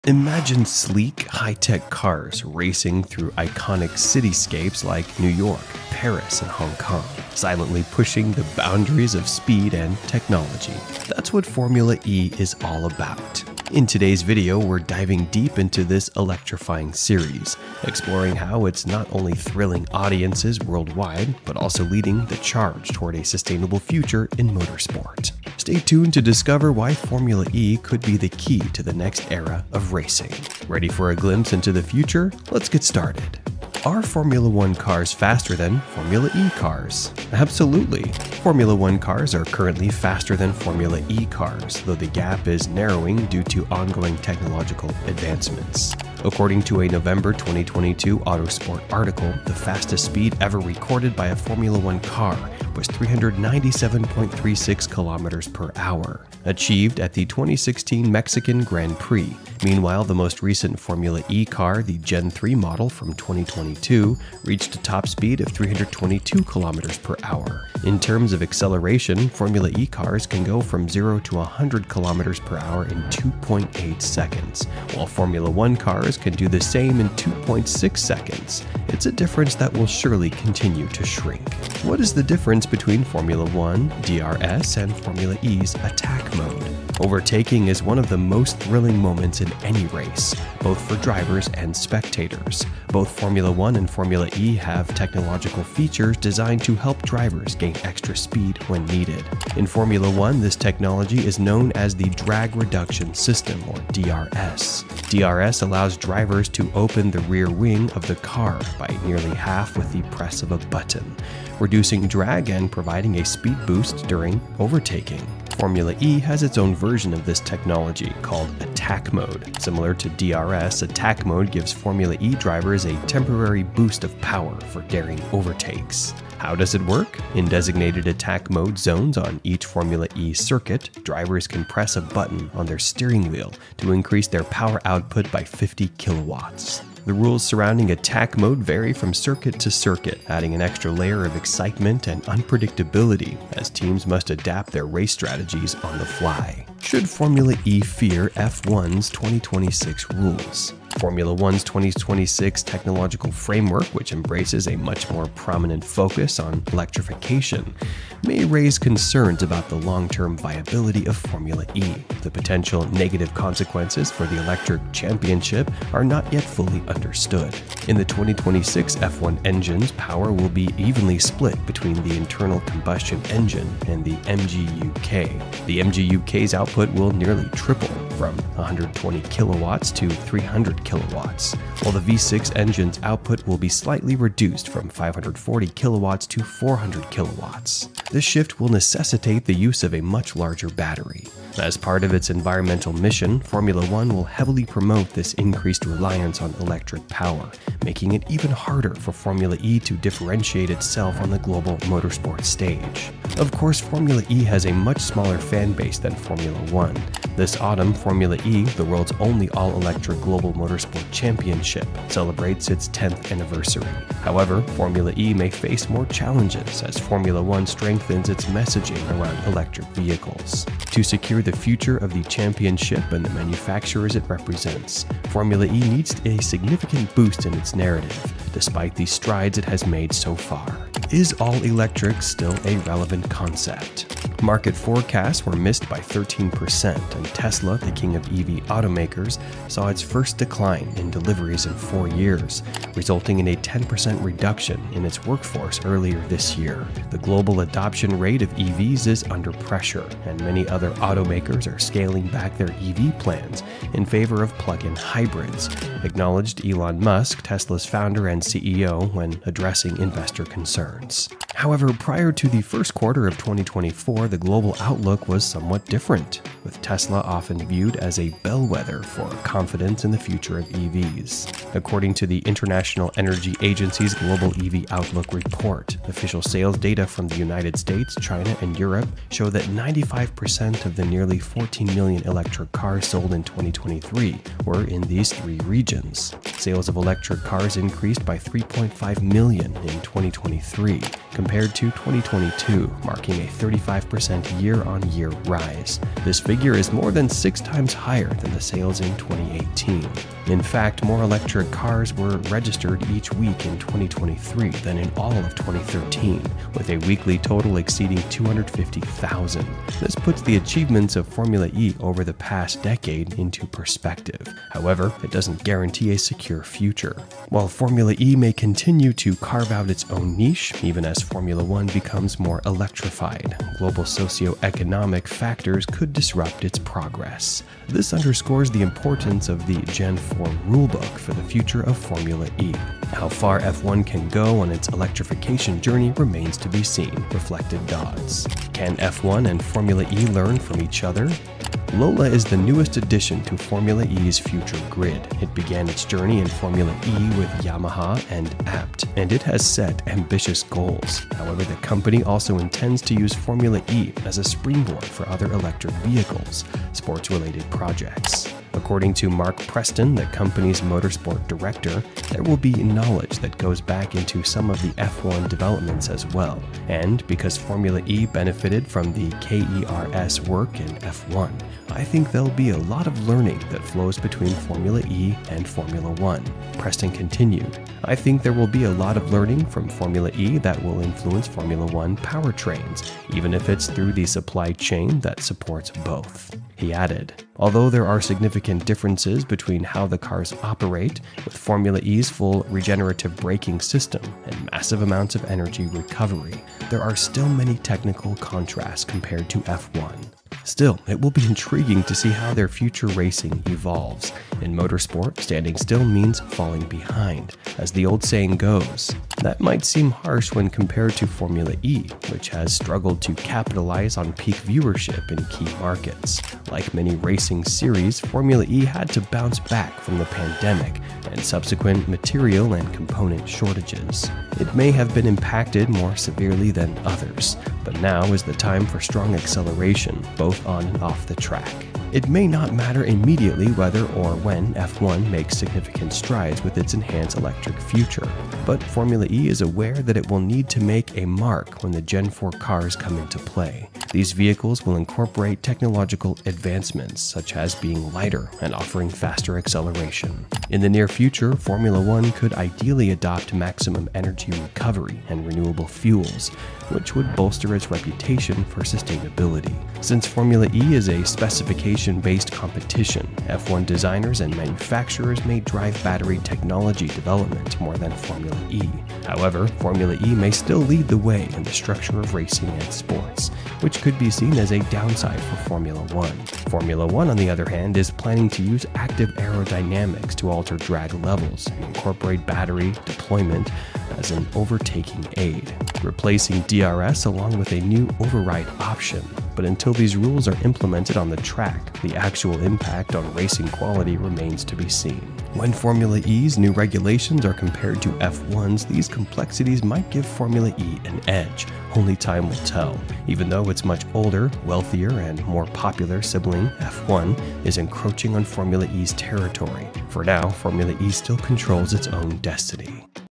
What is Formula E? The Future of F1?
Formula E is an all-electric racing series that showcases the future of motorsports, with its emphasis on sustainability, cutting-edge technology, and electrification. Launched in 2014, Formula E has emerged as a rapidly growing championship, offering an exciting alternative to the traditional fuel-powered Formula 1. It is designed to demonstrate the capabilities of electric vehicles while promoting eco-friendly technology, making it highly relevant in today's push for carbon reduction.

In Formula E, the cars are powered entirely by electric batteries, delivering instant torque and a thrilling race experience. While they may not yet match Formula 1 in terms of top speeds, Formula E cars bring unique challenges, such as energy management, efficiency, and handling on tight, urban circuits. This new form of motorsport takes place on street tracks in the heart of major cities, making it accessible to fans and providing a more immersive experience compared to the distant racetracks of F1.

As sustainability becomes a more urgent global priority, Formula E is paving the way for future innovations in racing, and its impact on F1 could be profound. Many believe that Formula 1 will eventually need to adopt hybrid or fully electric systems to stay relevant in a rapidly changing automotive world. While F1 remains the pinnacle of motorsport, Formula E represents the next frontier, combining thrilling competition with a mission to lead motorsport into a green, electric future.